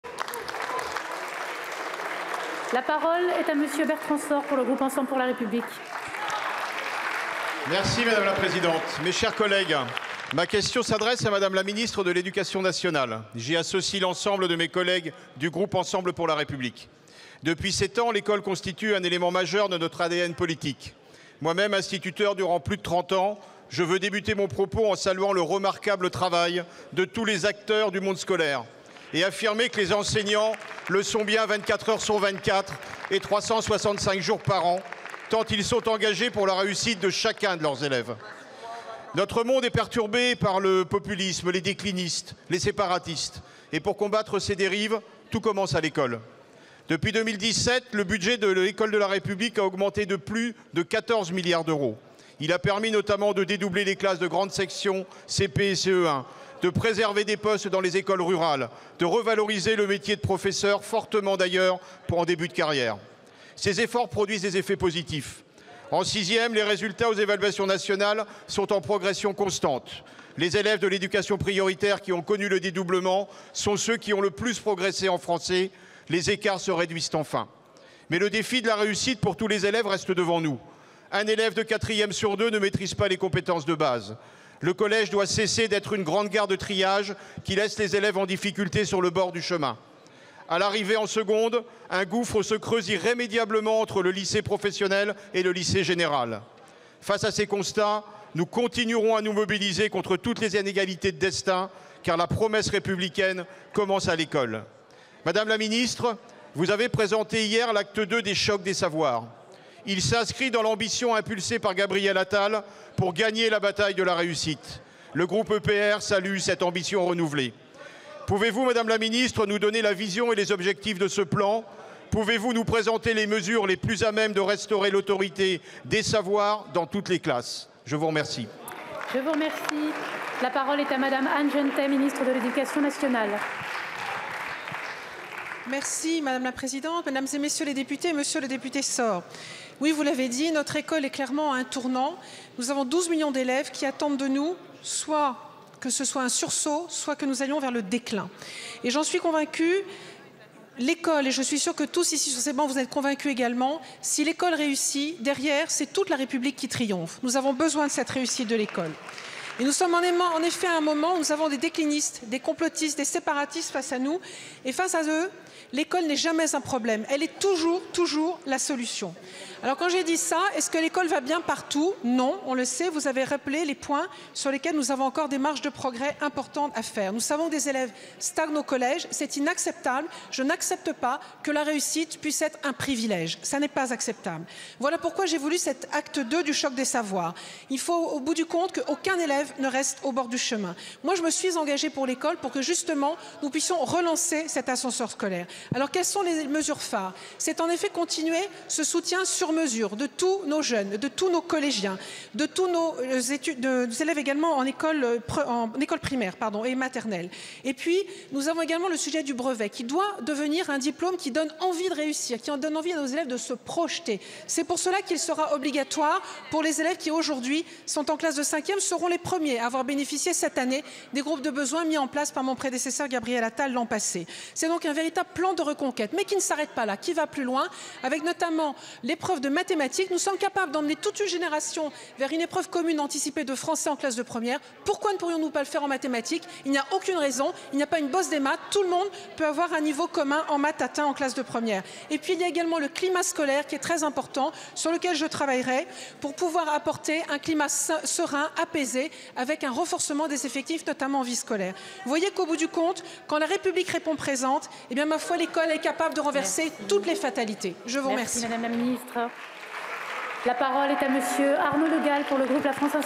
13/11/2024 QAG - Acte 2 du choc des savoirs (Education) - Bertrand Sorre
● TITRE :
QAG - Acte 2 du choc des savoirs (Education) - Bertrand Sorre

● CHAPITRES :
00:00:00 - M. Bertrand Sorre
00:02:10 - Mme Anne Genetet, ministre

● DESCRIPTION :
- Questions au Gouvernement

● DATE :
13/11/2024 13h43

● LEGISLATURE :
XVIIe législature